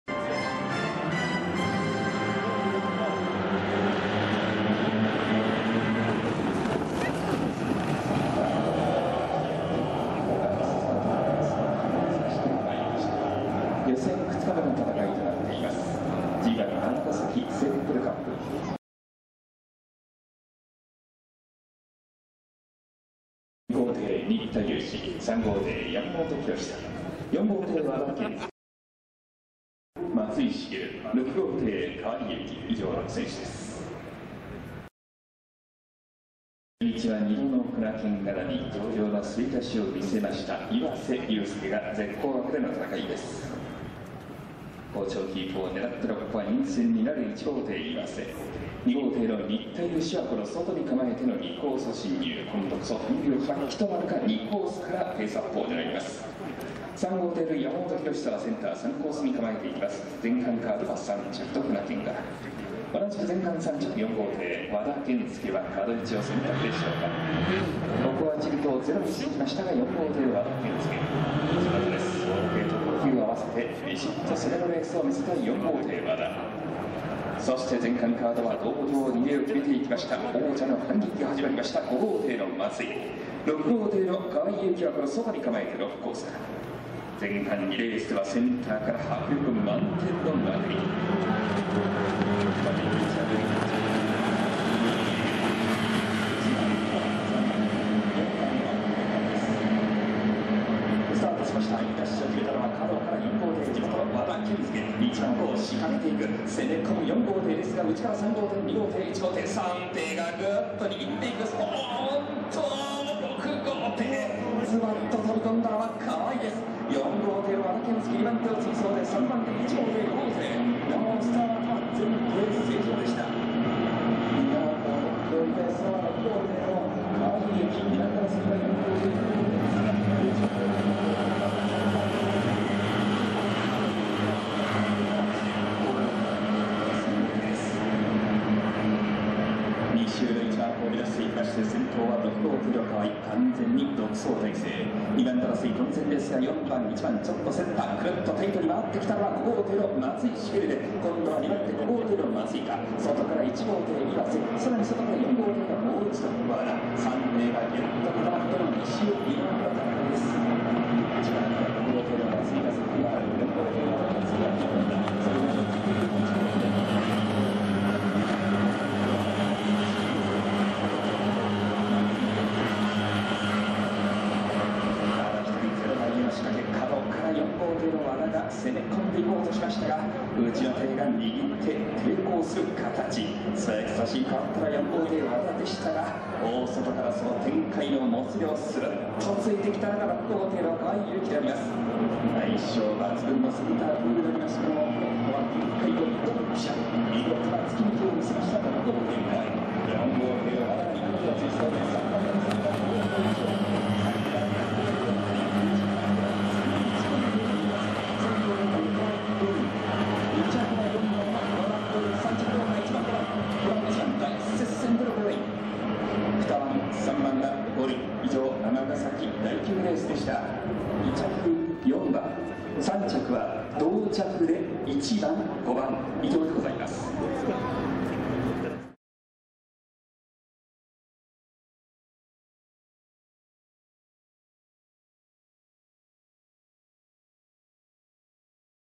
【現地】激戦！同着だ！松井繁選手VS岩瀬選手【センプルカップ・ボートレース尼崎】
2022年1月
ボートレース尼崎
G1　センプルカップ

2着～3着争いが激戦
最後まで松井繁選手と岩瀬選手が捌き合戦で同着での決着となりました